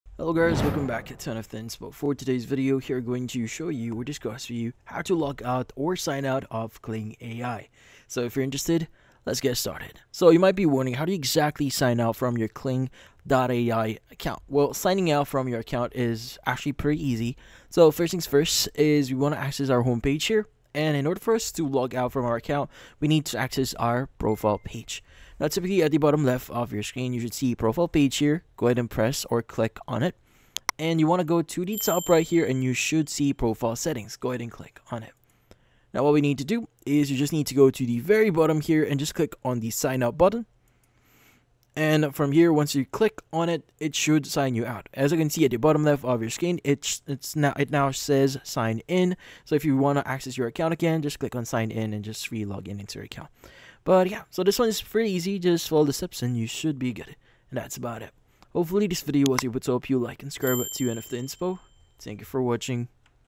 How To Logout/Sign Out Of Kling AI (2025)
If you want to be able to logout or sign out of kling ai, then this video will be perfect for you!